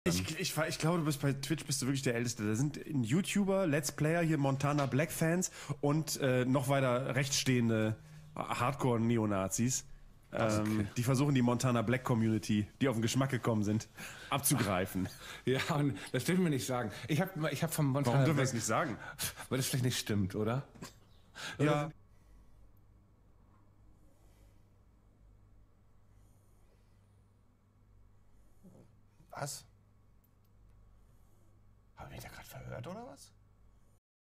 Böhmermann über MontanaBlack Fans : "HardCore Neo N*zis" 😡
Danke fürs Schauen eines Not N!ce Videos! Auf unserem Channel findet ihr viel mehr Zusammenschnitte verschiedener Streamer & C.o! Schaut doch mal vorbei ;)